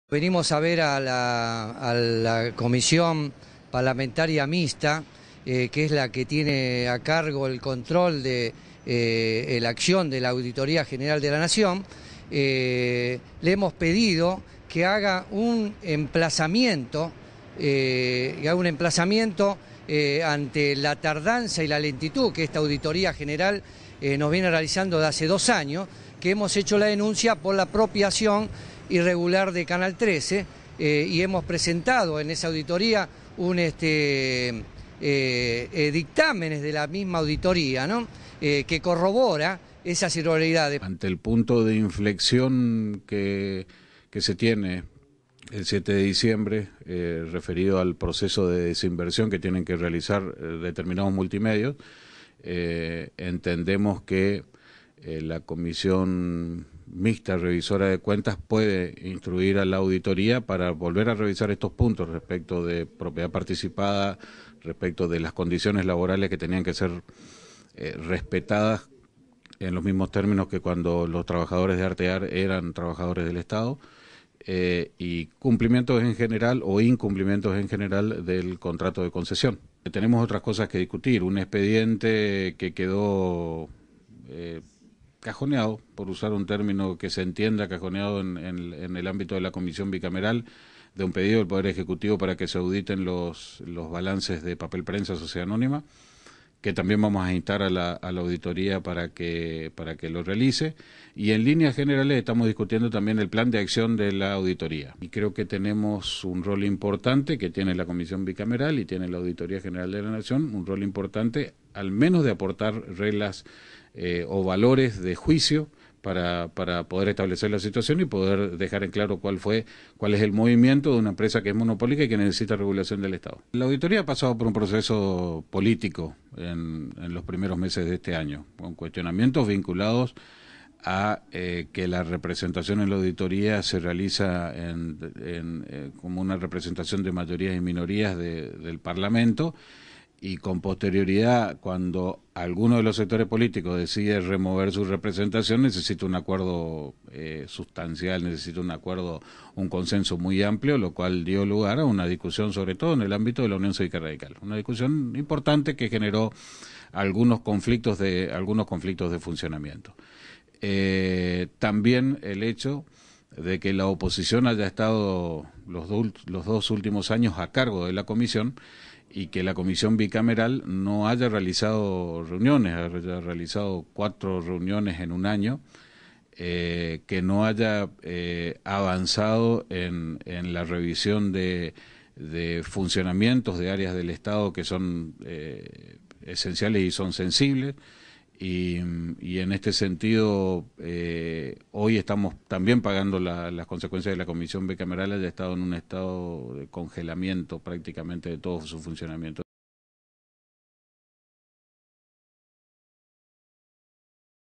Carbonelli - Ríos
Instan a la AGN a que audite los balances de Papel Prensa y posibles irregularidades en la concesión de Canal 13
El Diputado Nacional y  presidente de la comisión parlamentaria mixta revisora de cuentas, Fabián Ríos, anunció que le pedirán a la Auditoria General de la Nación (AGN) que revise los balances de la empresa Papel Prensa y también posibles irregularidades en la concesión de Canal 13, luego de una reunión con el ex delegado José Carbonelli.
"Tenemos un rol importante para poder dejar en claro los movimientos de una empresa que es monopólica y que necesita regulación del Estado" declaró el legislador correntino por el Frente para la Victoria. A su vez, criticó el "congelamiento que sufrió la comisión en los últimos dos años, cuando estuvo a cargo de la oposición".
Por su parte, el ex delegado de Canal 13, José Carbonelli, explicó que han "pedido que hagan un emplazamiento ante la lentitud de la Auditoría General de la Nación en la denuncia de apropiación irregular de Canal 13".